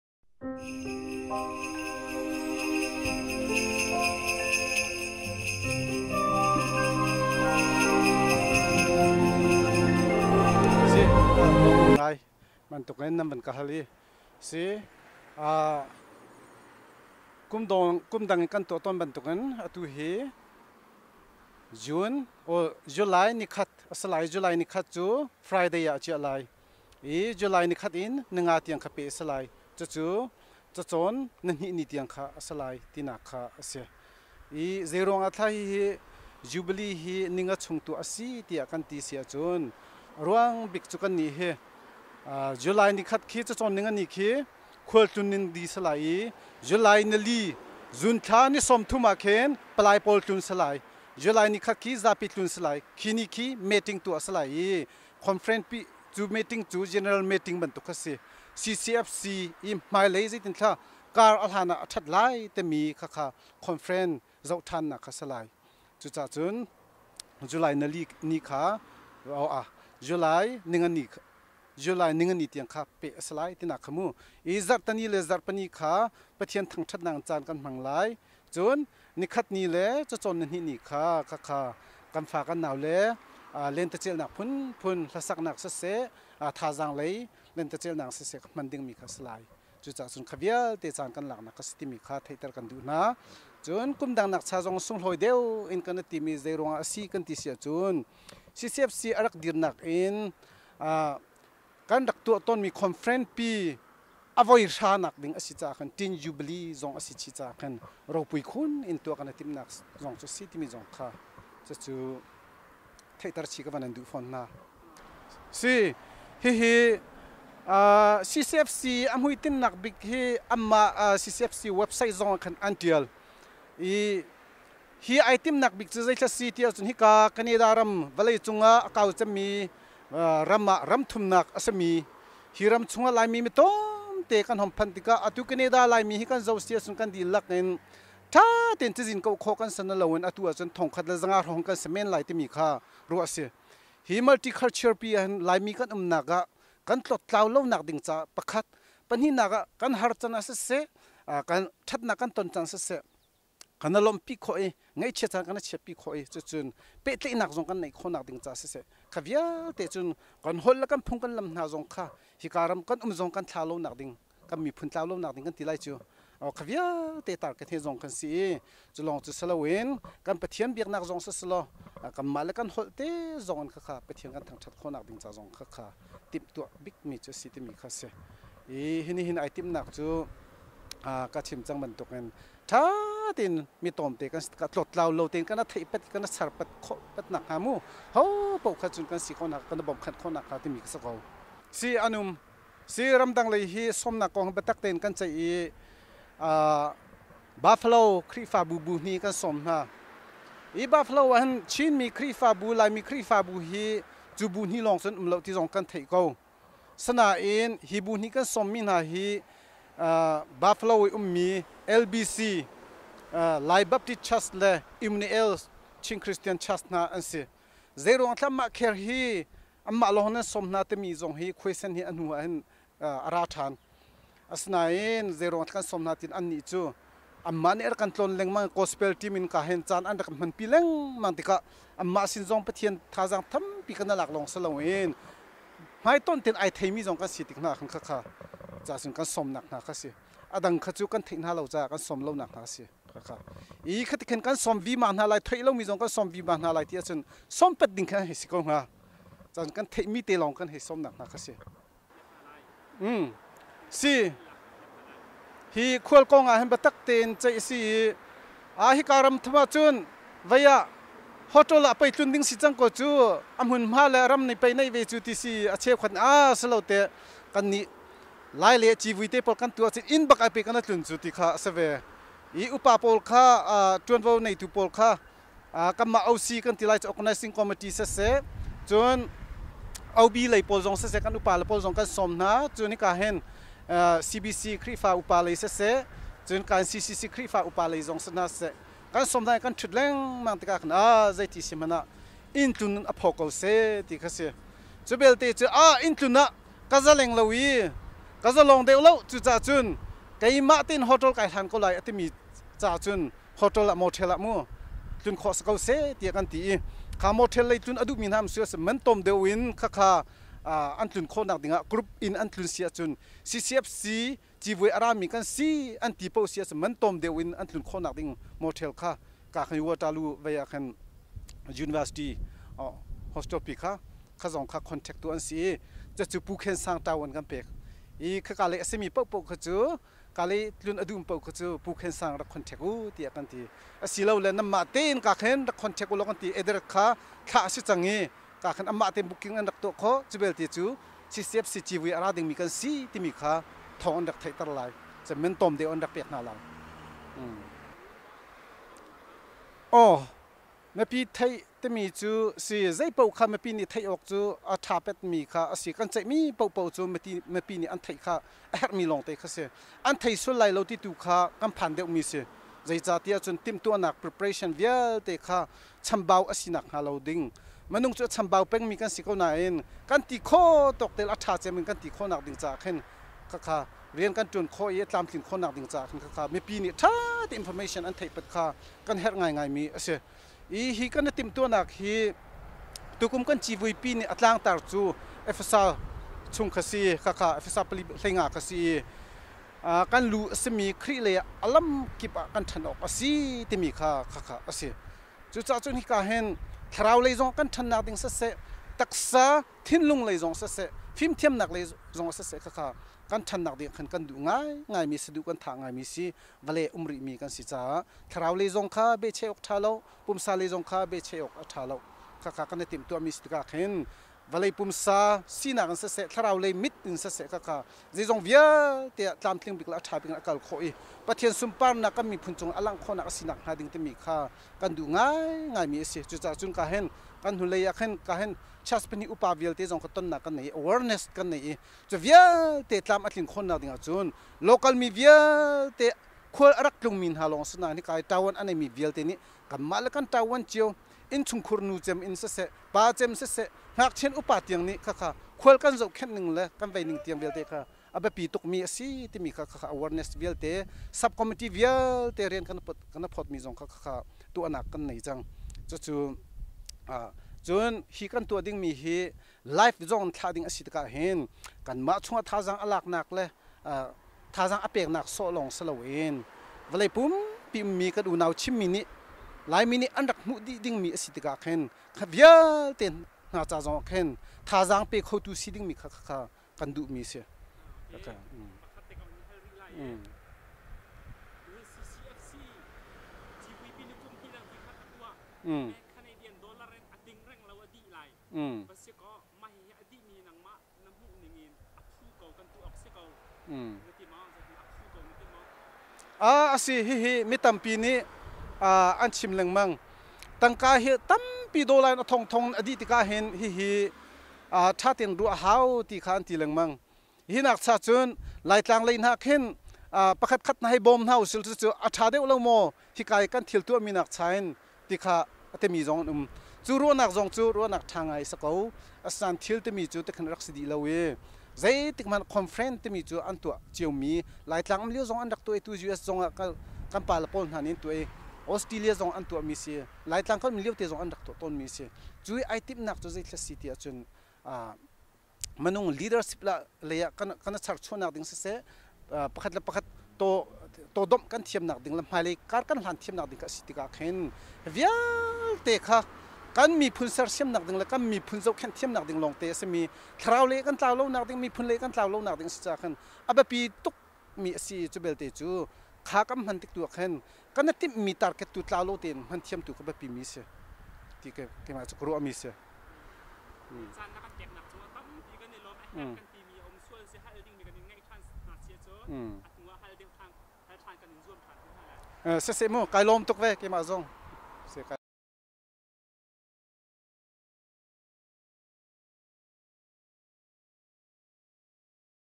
Pu Cung Lian Bik CCFC Civui Tuahnak Ding Biaruahnak
2016 CCFC Civui Local  Organization Committee chairman Pu Cung Lian Bik, biatawi ruahnak